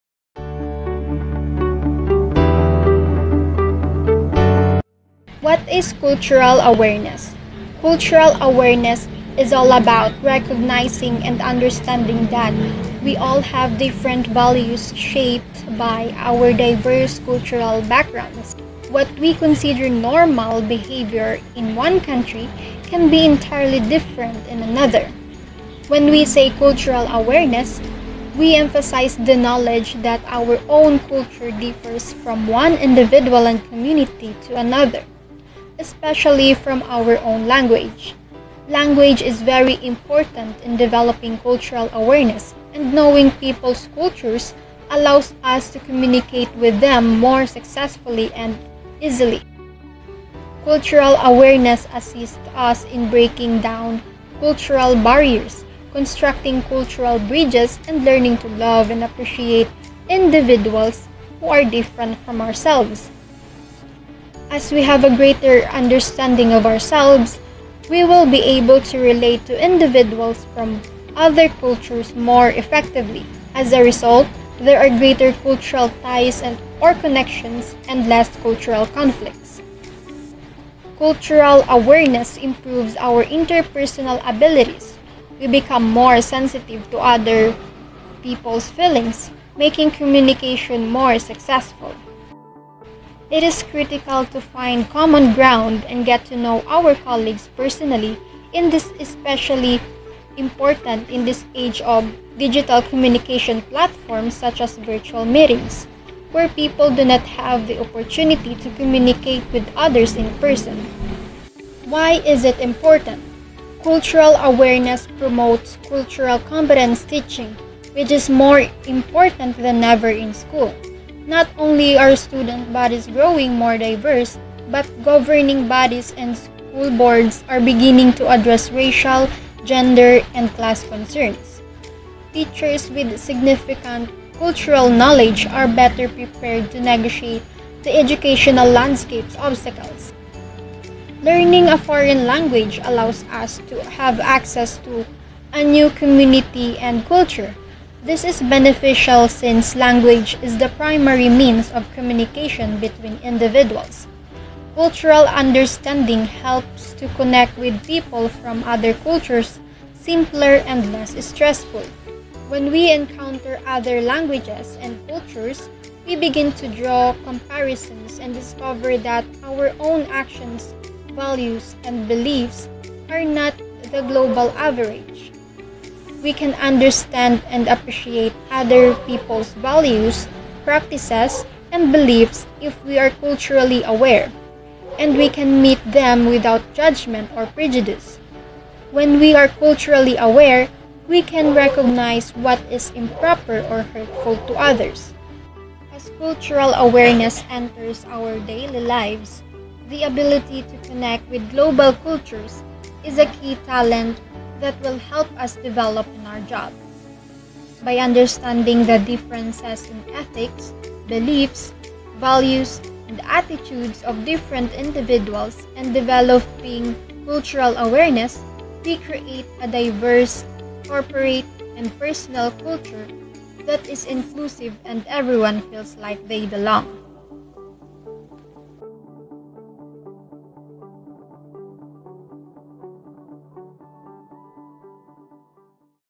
ADVOCACY SPEECH | CULTURAL AWARENESS | Purposive Communication
ADVOCACY SPEECH | Cultural Awareness

Prepared by: Cydny Mae L. Loresto
Submitted to: Prof. Roma S. Abin

REFERENCES:

Faleti, Y. (2017, April 18). News - how cultural awareness promotes effective communication. News - How Cultural Awareness Promotes Effective Communication | Stevenson University.

House, M. S. (2019, August 19). Cultural awareness. Macmillan Education: ELT courses, digital solutions, and educational materials.

Importance of Cultural Awareness for Educators. Mississippi College Online. (2021, February 4).

Maina, D. (2021, December 5). News - how cultural awareness promotes effective communication. News - How Cultural Awareness Promotes Effective Communication | Stevenson University.